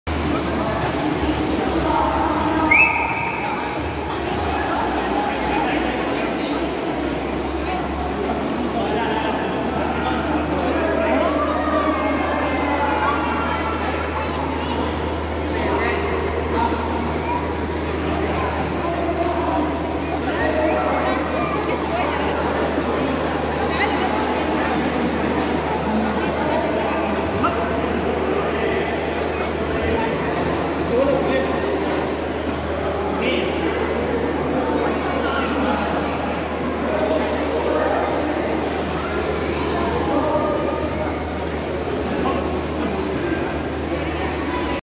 Swimming training classes for children Tabuk Level 01 Tabuk Saudi Arabia
Swimming training classes for Childerens Tabuk Level 01 beginners level one for Children Hamza And Rayyan Also started